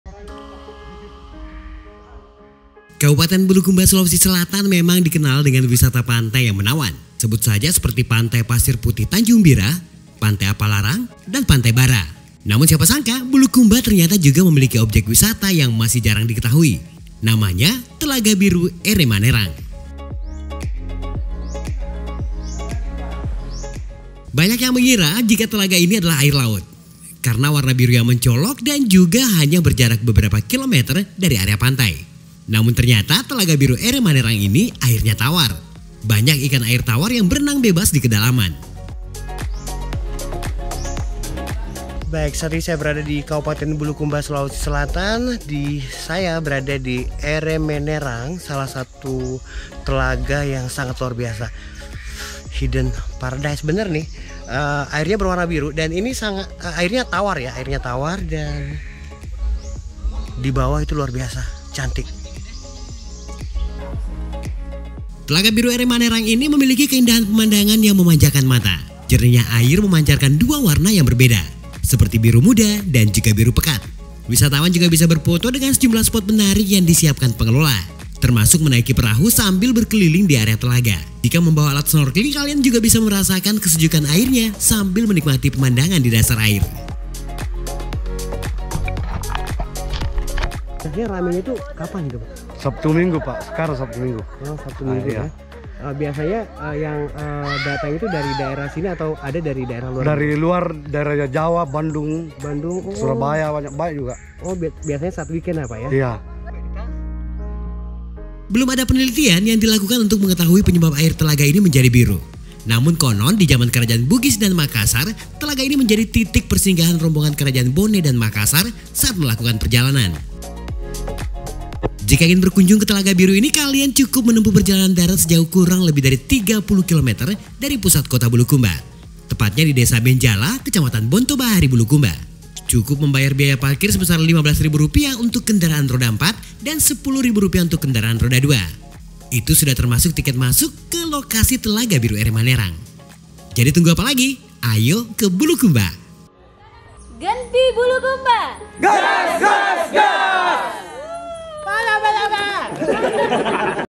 Jarang Ada yang Tahu, Bulukumba Punya Telaga Biru Tempat Mandi Keluarga Raja
Banyak yang mengira jika air telaga ini merupakan air laut karena warna biru yang menyolok dan hanya berjarak beberapa kilometer dari area pantai. Namun ternyata Telaga Biru Ere Manerang ini airnya tawar. Banyak ikan air tawar yang berenang bebas di dalamnya.